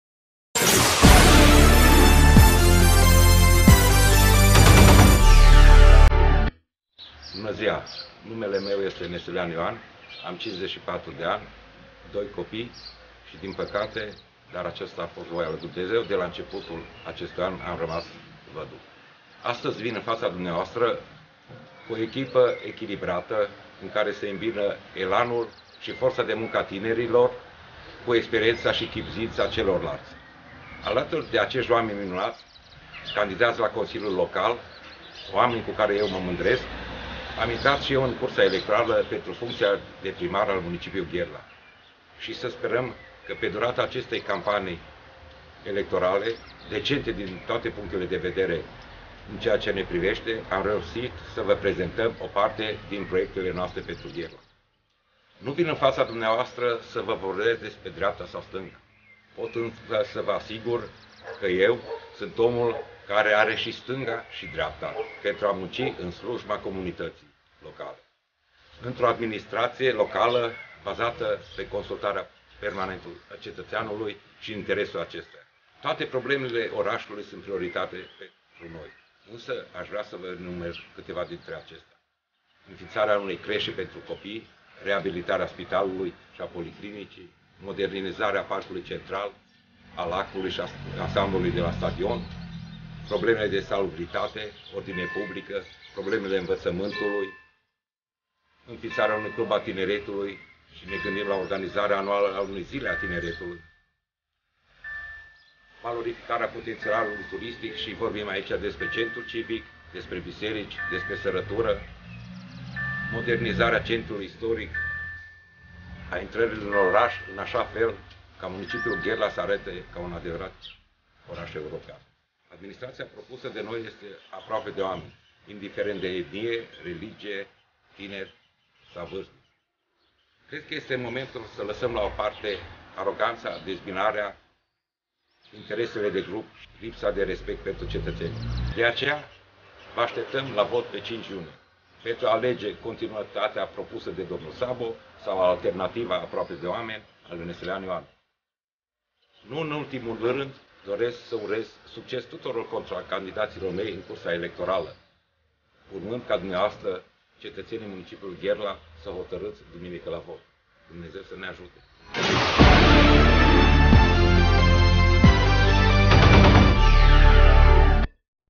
Ioan Neselean candidatul PSD la primaria Gherla (Cluj)
Mesajul candidatului PSD Ioan Neselean la primaria Gherla pentur alegatori - vezi mai mult aici